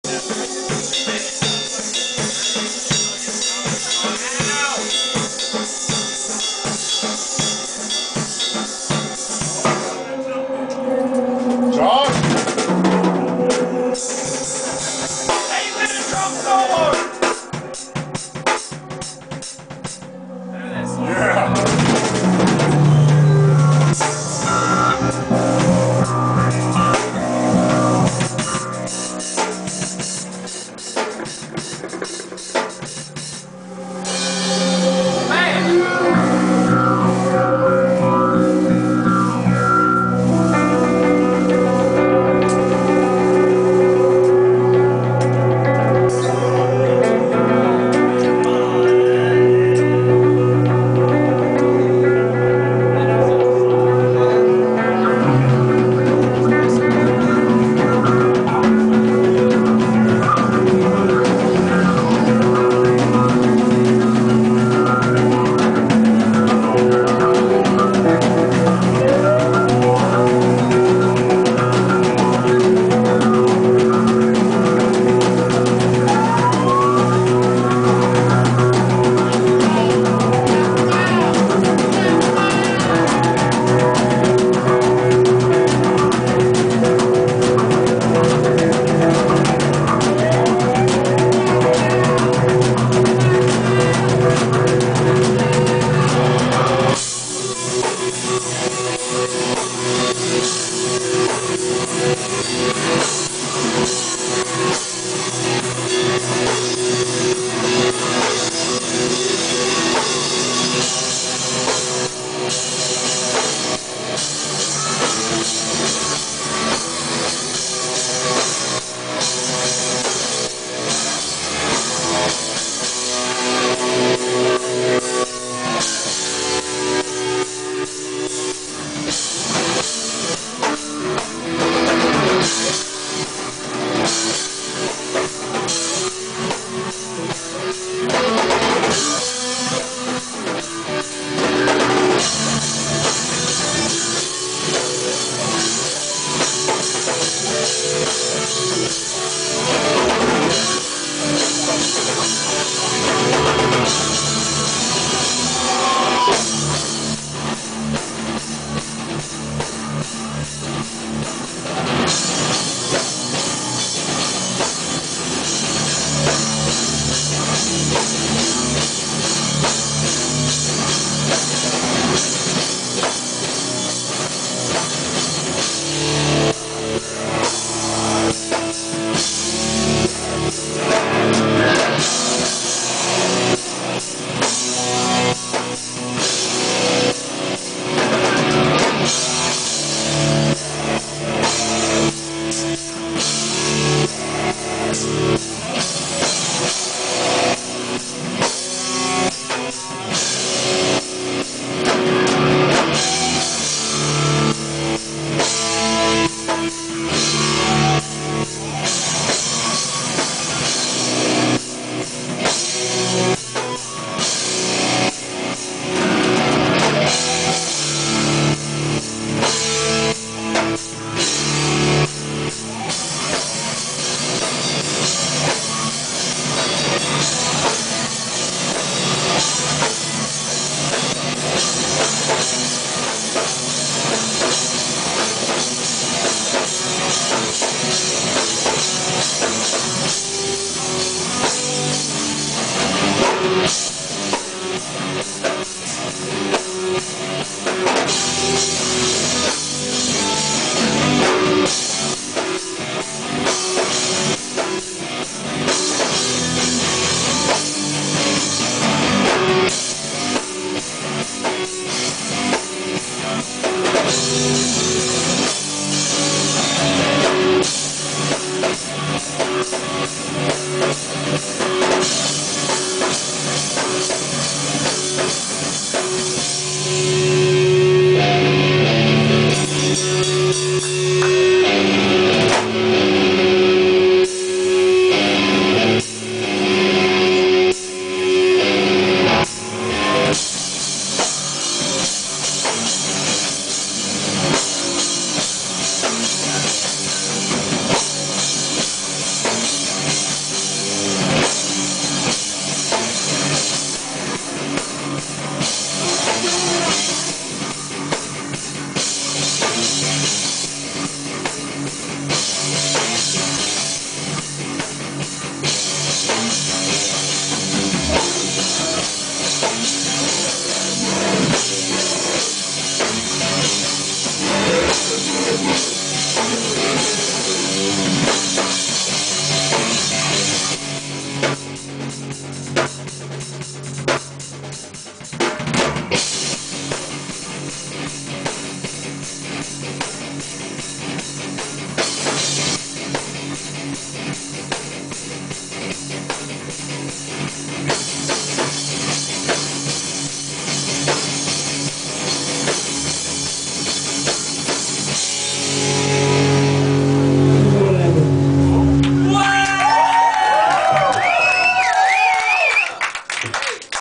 Moose live @ 1126 House 2/13/10
Moose performing live at 1126 House in Hattiesburg, MS. Record 2/13/10.